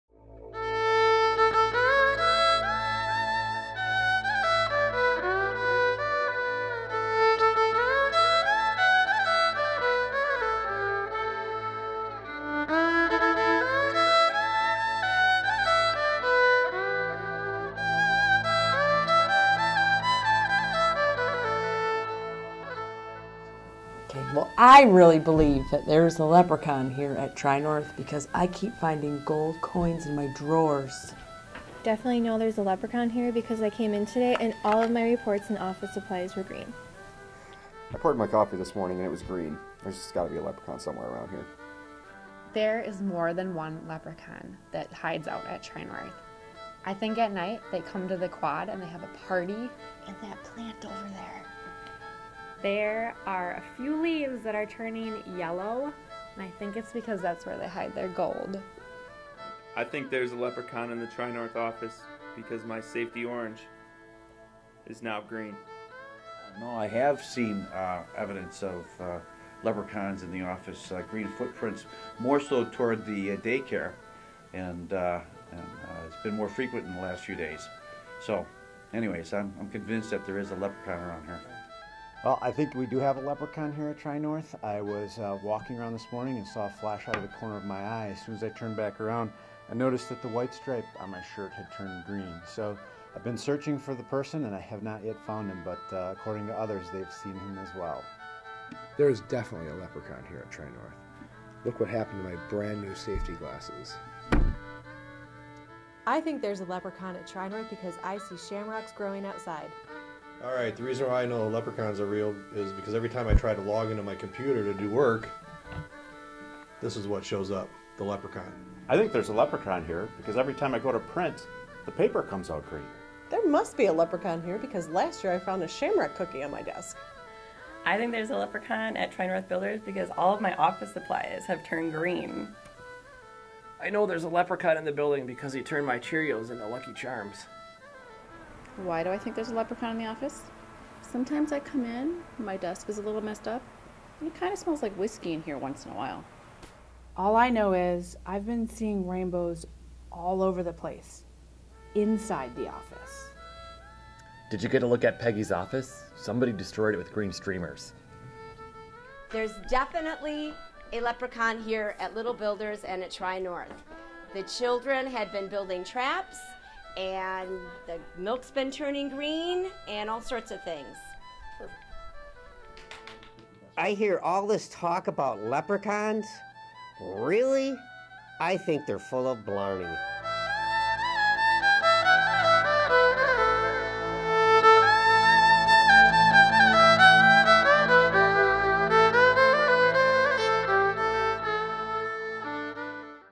Happy St. Patrick's Day 2016
Two years ago, our Little Builders discovered a brilliant, magical rainbow winding through our office. They eagerly set out to skip along its colorful path and discovered a leprechaun in "The End of the Rainbow."  Is this leprechaun real? Does he reside at Tri-North? We asked our employees to weigh in this year and asked them "Are Leprechauns Real?"